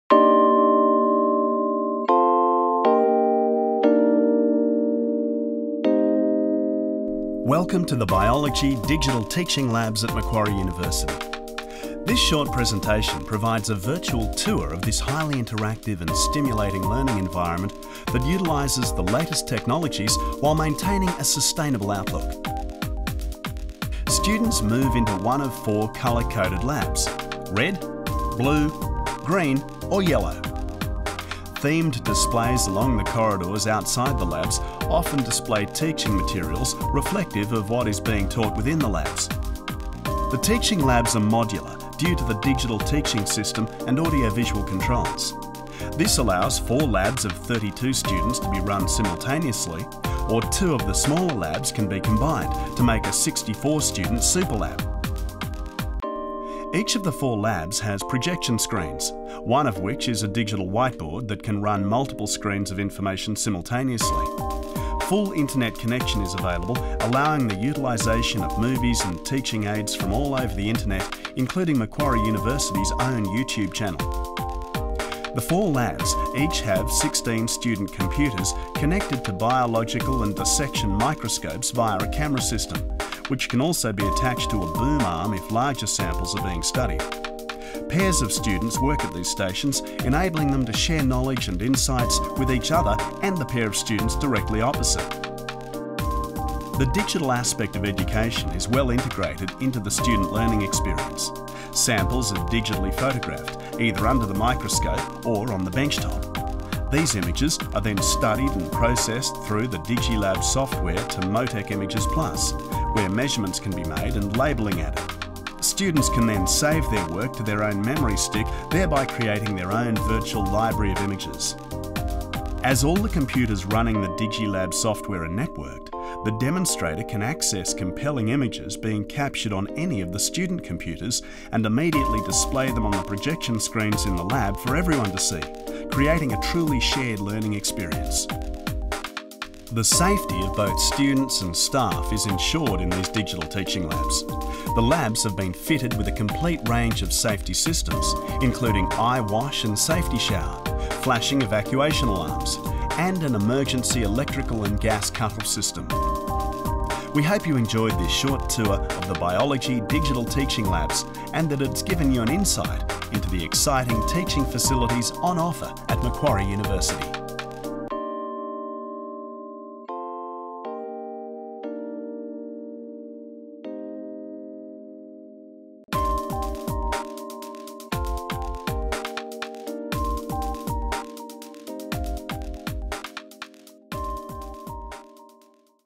Macquarie University Biological Sciences Lab Tour
Take a tour of the cutting-edge Biology Digital Teaching Laboratories at Macquarie University. With state-of-the-art teaching systems and computerised microscopes our students benefit one of the finest learning environments.